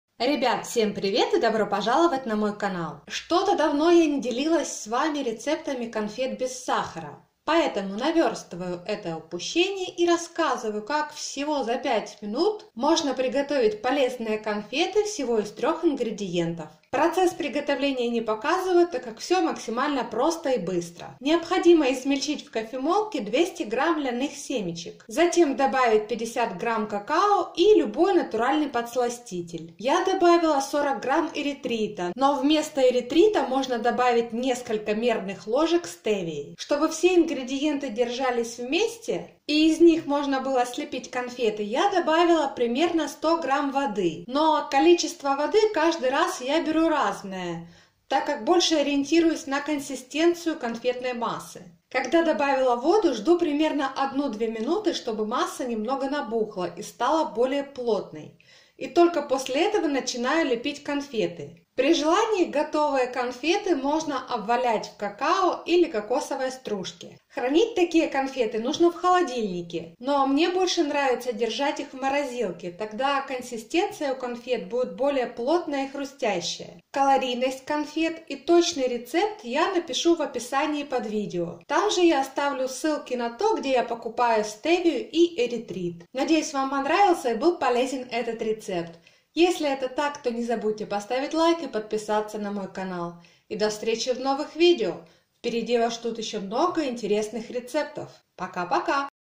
Полезные сладости без сахара | Рецепт конфет из семян льна
Как приготовить полезные диетические конфеты без сахара и с пониженным содержанием углеводов.

Для приготовления полезных сладостей понадобится:
• Семена льна – 200 г
• Какао – 50 г
• Натуральный подсластитель, например, эритрит или стевия.

В кофемолке измельчить льняные семечки, добавить какао и подсластитель. Перемешать сухие ингредиенты и влить примерно 100 г воды. Воды может понадобиться чуть больше или чуть меньше, ориентироваться нужно на консистенцию массы, она должна быть плотной и хорошо держать форму. Слепить конфеты в виде шариков. Готовые конфеты можно обвалять в какао или кокосовой стружке.

Конфеты из семян льна: на 100 грамм калорийность – 502 ккал, белки – 19.48, жиры – 37.26, углеводы – 29.5.

💡 AFE9825 - код на скидку 10% на весь заказ на сайте Iherb